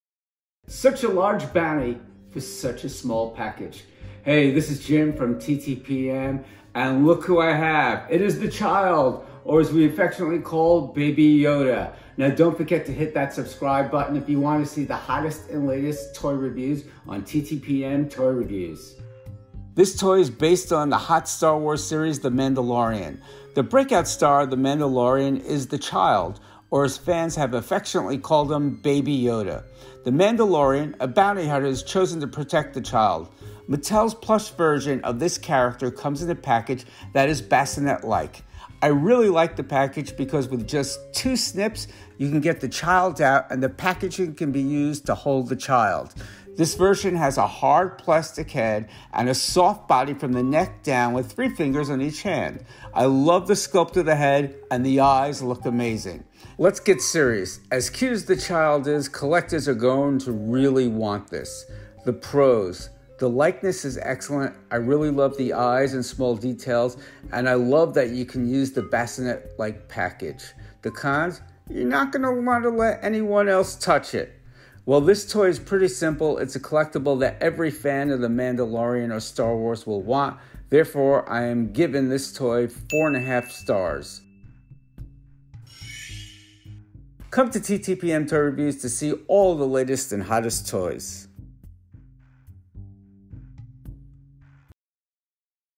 Toy Review | Star Wars The Mandalorian The Child Plush from Mattel | Better known as Baby Yoda
Star Wars The Mandalorian The Child Plush Stuffed toy that is affectionally know as Baby Yoda. For full review and shopping info

Product Info: This toy is based on the hot series Star Wars, The Mandalorian. The breakout Star of the Mandalorian is The Child, or as fans have affectionally called him, Baby Yoda. The Mandalorian, a bounty hunter has chosen to protect The Child. Mattel's plush version of this character comes in a package that is bassinet like. We really like the package because with just two snips, you can get The Child out and the packaging can be used to hold The Child. This version has a hard plastic head, and soft body from the neck down with 3 finger on each hand . We love the sculpt of the head, and the eyes look amazing.